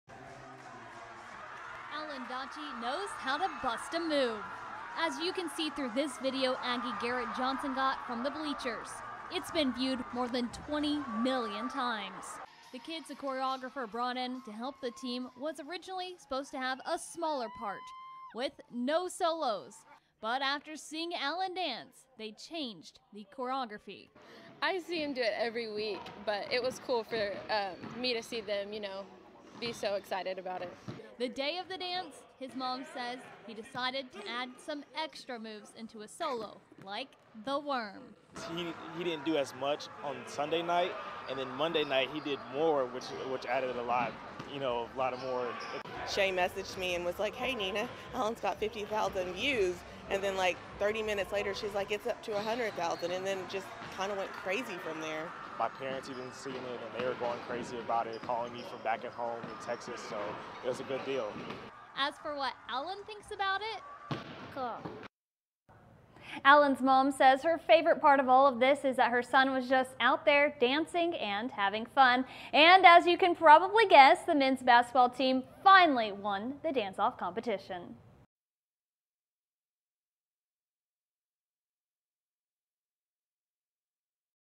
Lawton kid’s solo at Cameron’s Aggie Madness goes viral
LAWTON, OK (RNN Texoma) - A kid from Lawton is going viral after he stole the show at Cameron University’s Aggie Madness. The pep rally was Cameron’s way to kick off the basketball season. During Aggie Madness, the men’s and women’s basketball team have a dance-off competition, and the women’s team has won at least the last four years in a row. Because of that, some members of the men’s basketball team decided to ask a friend to choreograph a dance, and she brought in some of her students.